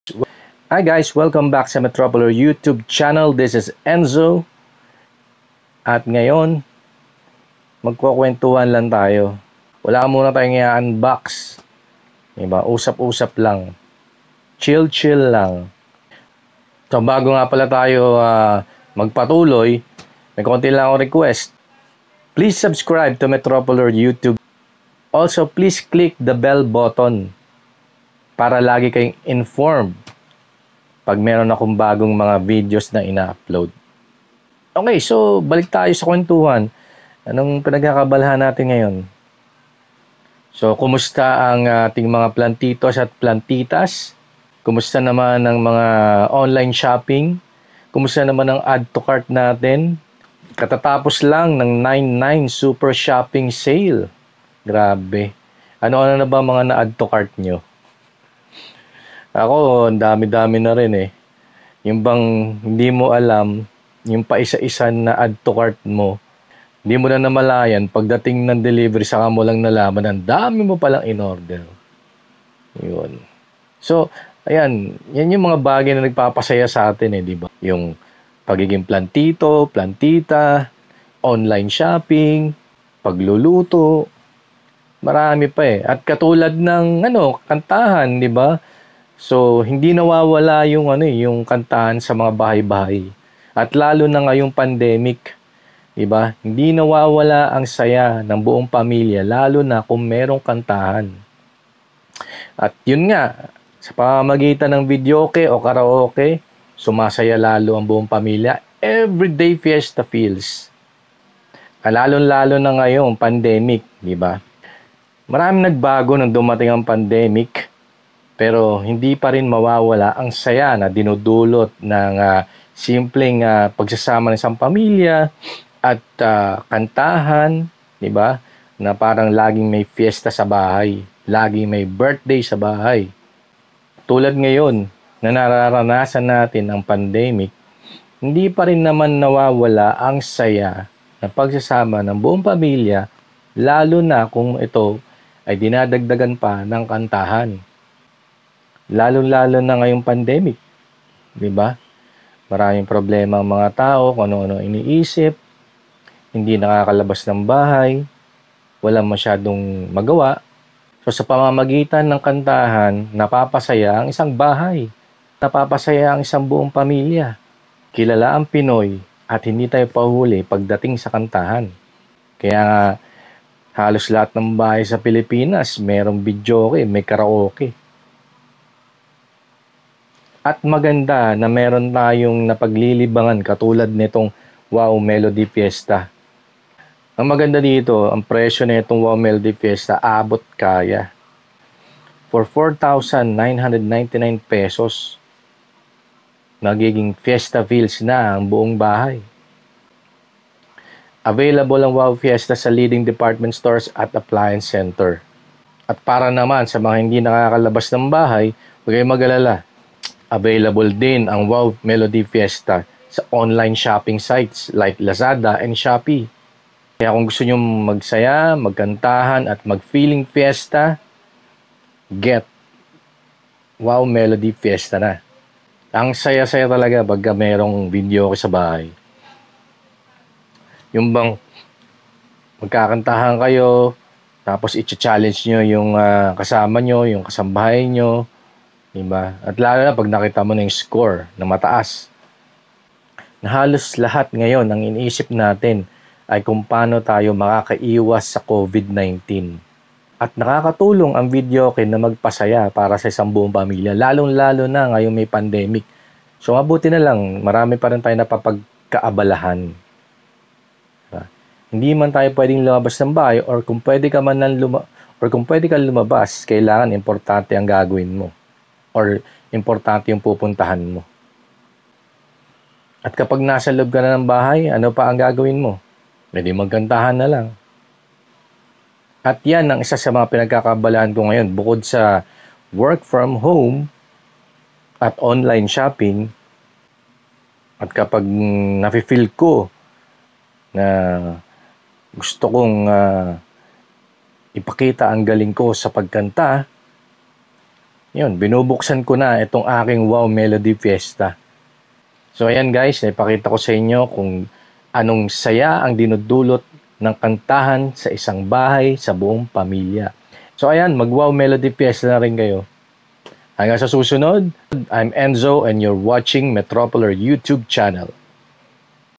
KWENTUHAN LANG: VIDEOKE DURING THE PANDEMIC
Filipinos are known for their love of music and singing. Mahilig ang Pinoy sa videoke and eto ang isa sa pampalipas oras at pinagkakaabalahan ng mga tao ngayong may pandemic.